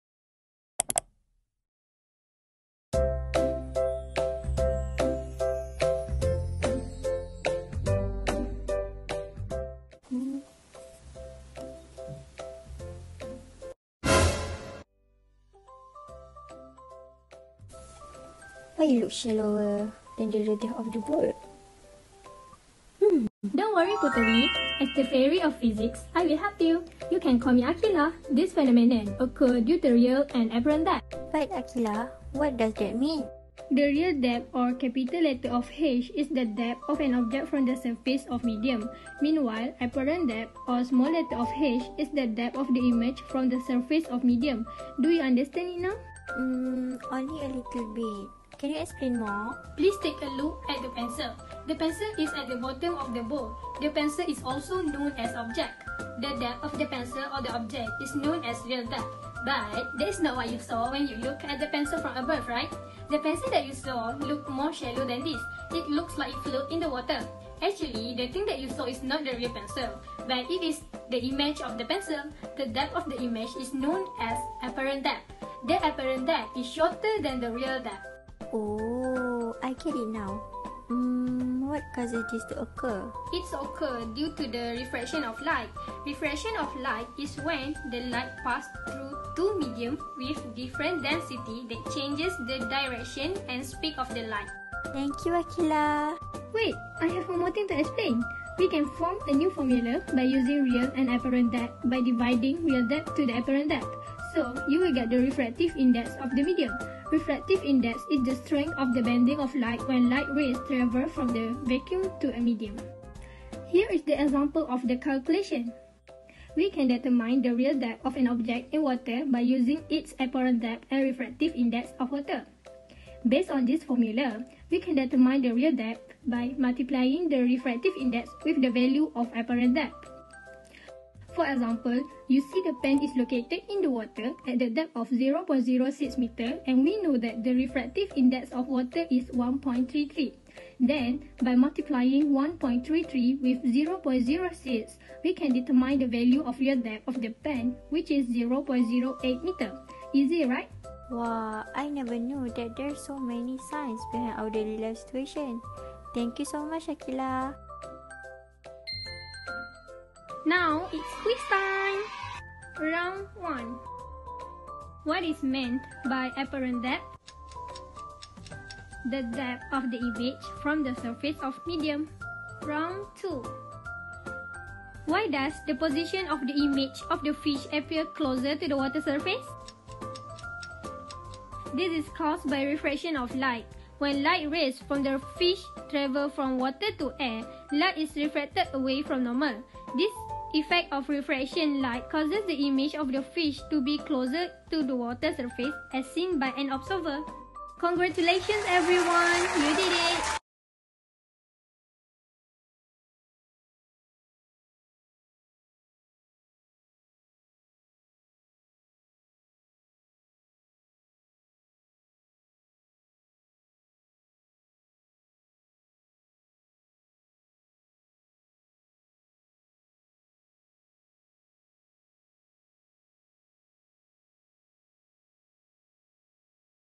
SESMA OUTREACH 2021: FIZ 5 - REAL & APPARENT DEPTH
PHYSICS GROUP 5

MEMBERS:
1. NURSHAZA SYAUQINA BINTI NURSHARULHAMI
2. PUTERI ALYA NABILAH BINTI AMINUDDIN
3. SITI NUR AQILAH BINTI ASMADI
4. WAN ASLAH HAKIM BIN WAN MOHD ROJI
5. AFIFAH ZAHRA BINTI ZULKIFLI
6. AHMAD DANIAL ADLI BIN MOHD ZAKI

MENTOR:
DR. SUHAIZA BINTI SAID

TITLE:
REAL & APPARENT DEPTH